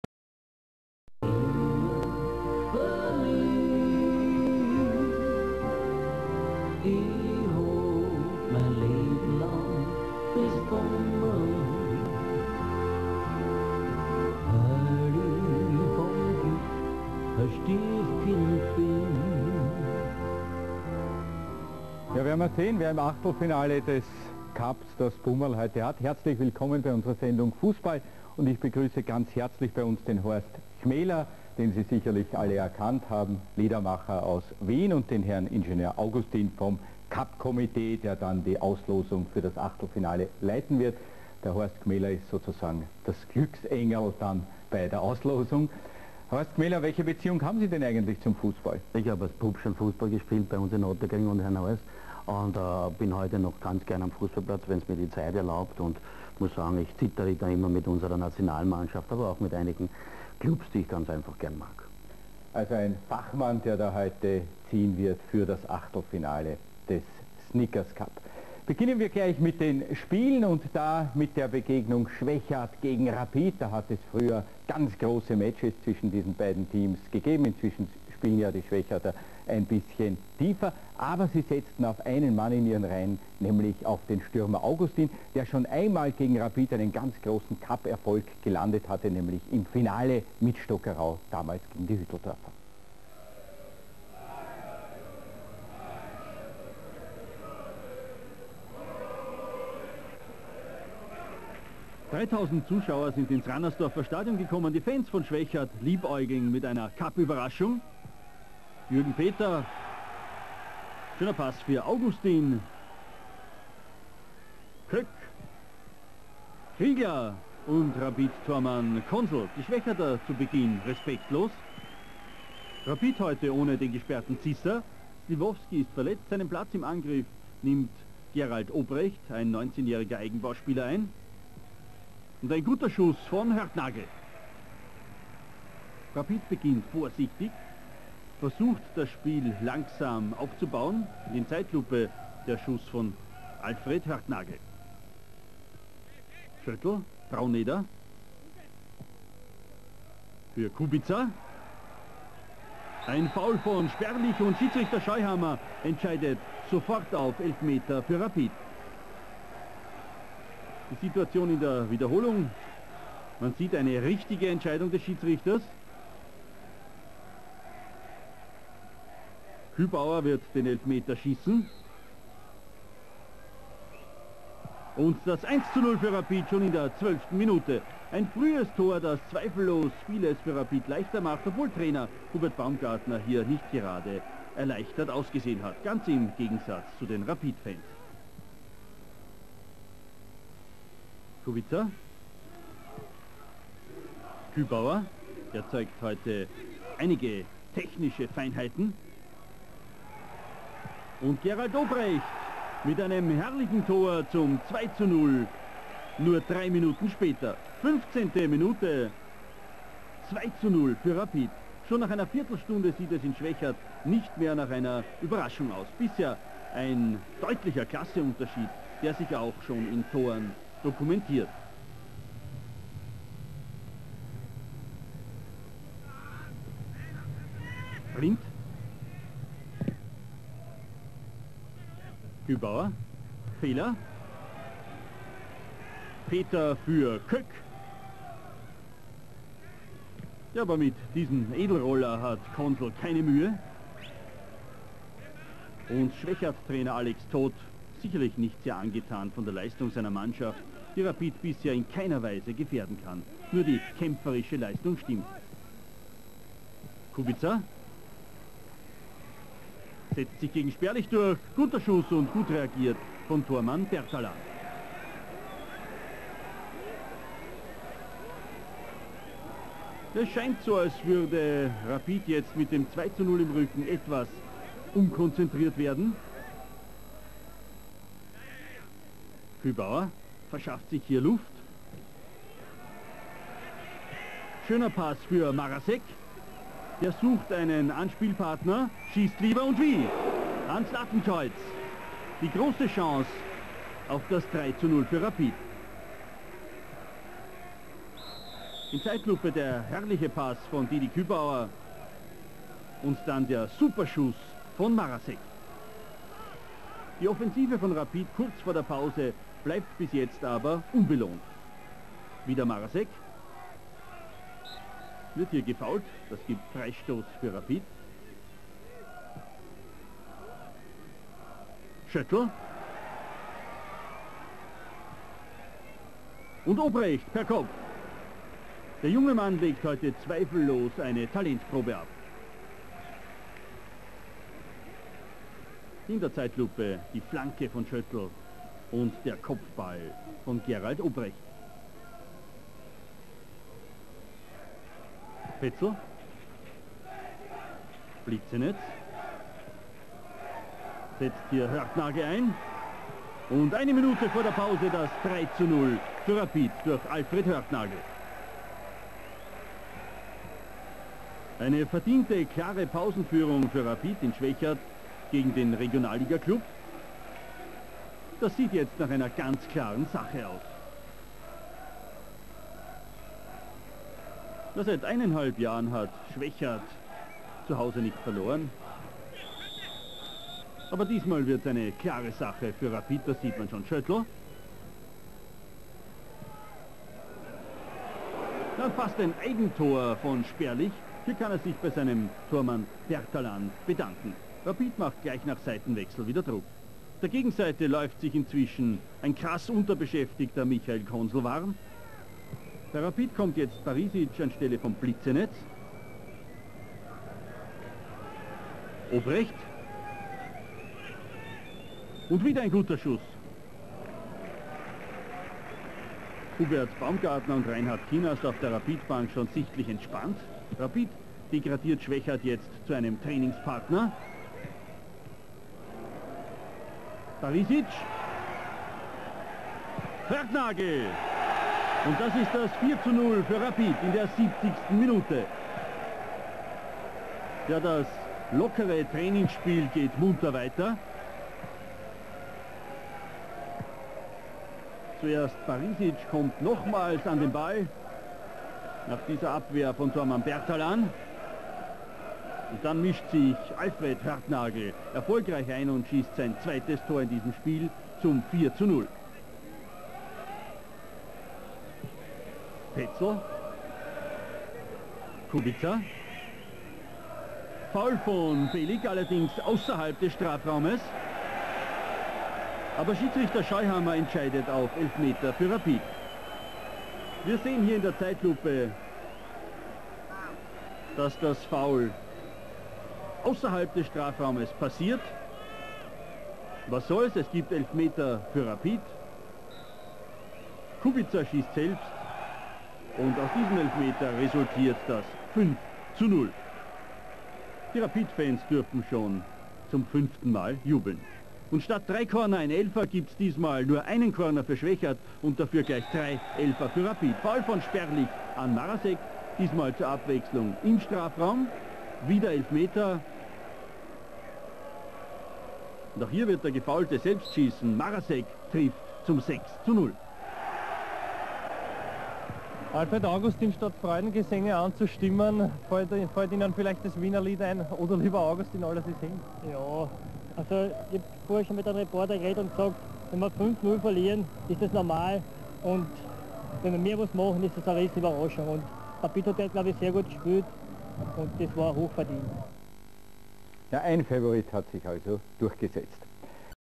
SV Schwechat - Rapid Wien 0:6 - 3. Cup Runde 1993/94
Cup 1993/94 - Sechzehntelfinale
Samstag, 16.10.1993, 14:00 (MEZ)
Schwechat, Rannersdorf, 3000 Zuschauer
Schiedsrichter: Johann Scheuhammer (Österreich)

SV SCHWECHAT 0:6 (0:3) RAPID WIEN
Tore:

0:1
Dietmar Kühbauer (Elfmeter) 12'

0:2
Gerald Obrecht 15'

0:3
Alfred Hörtnagl 44'

0:4
Alfred Hörtnagl 70'

0:5
Andrzej Kubica (Elfmeter) 78'

0:6
Stefan Marasek (Elfmeter) 90'

Aufstellungen
Coach: Alex Toth

Coach: Hubert Baumgartner
Bertalan

Michael Konsel
Frind

Peter Schöttel
Sperlich

Franz Blizenec
Belik

Robert Pecl
Peter

Karl Brauneder
Köck

(73' Patrick Jovanovic)
Kriegler

Alfred Hörtnagl
Stastny

Stefan Marasek
Schneider

Horst Steiger
(11' Pinka)

Dietmar Kühbauer
Schober

Gerald Obrecht
(73' Kultscher)

Andrzej Kubica
Augustin